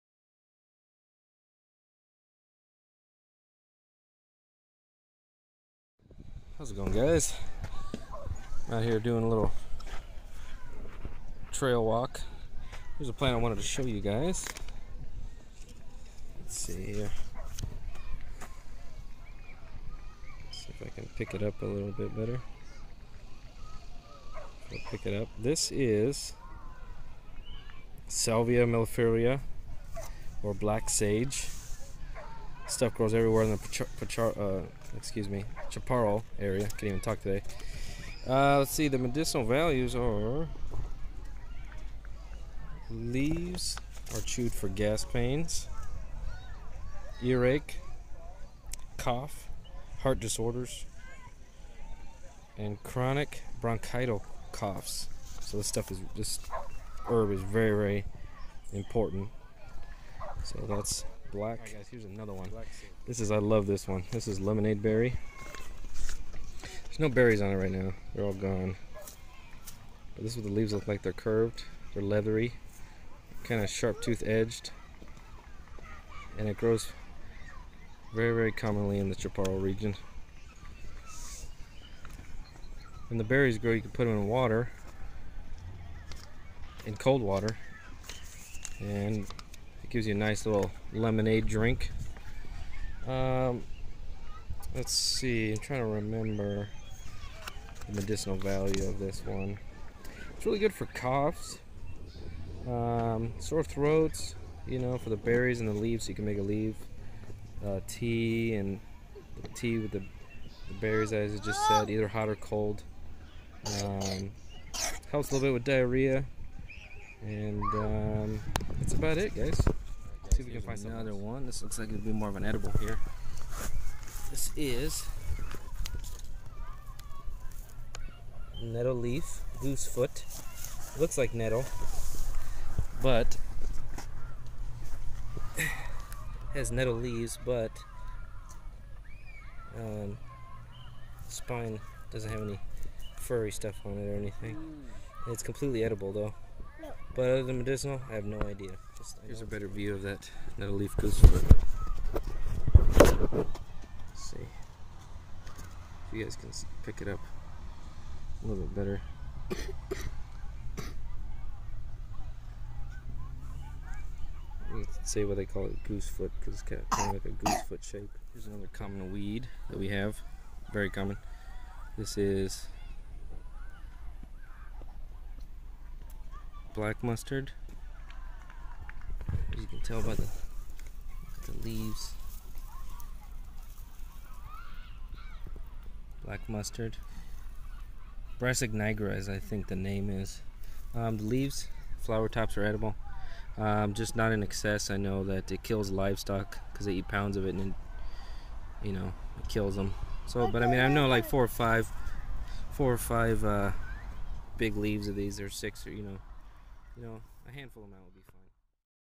WILD EDIBLE AND MEDICINAL PLANTS - SERIES 1
in this video I show a few edible and medicinal plants of California explaining their uses